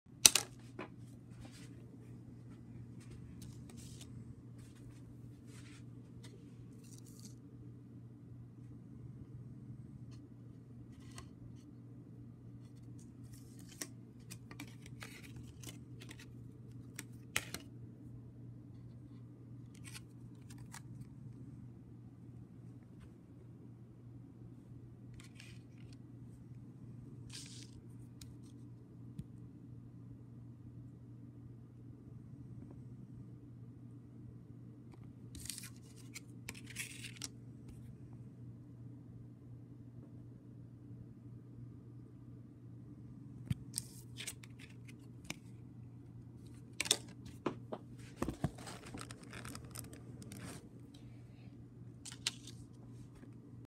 I found a old CTA Ventra card!
This is very old Ventra card and this is CTA card and I don’t own this CTA Ventra card and I film Ventra tickets and I say Freedom Tower Studios go check out my video and hope you guys like this video!